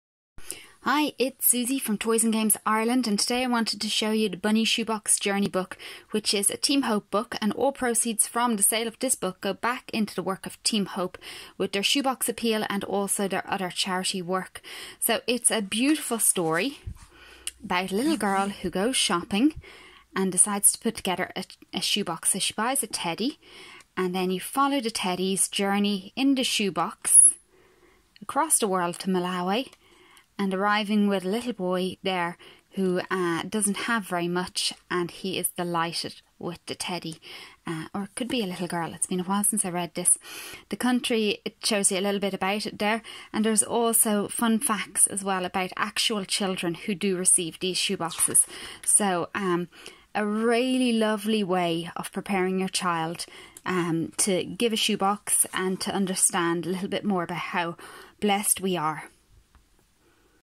Bunny's Shoebox Journey
This book is published by Team Hope Ireland. It is designed for preschool and primary school children to show them the benefit of sending a Christmas Shoebox to boys and girls around the world. You can buy this children's book from Toys and Games Ireland